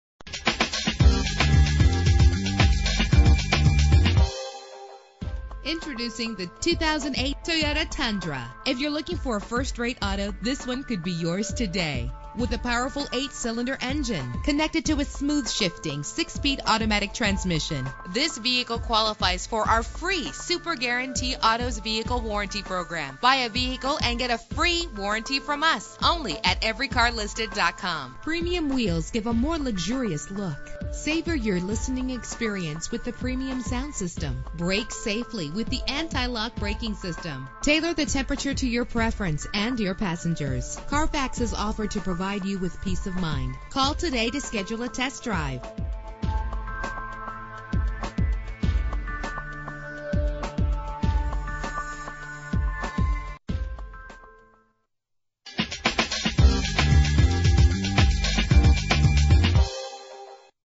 2008 Toyota Tundra - Little Rock AR
Year: 2008
Make: Toyota
Model: Tundra
Trim: Limited
Engine: 5.7 liter 8 cylinder 32 valve
Transmission: 6-Speed Automatic
Color: Red
Mileage: 34655
Address:
10825 Colonial Glenn Road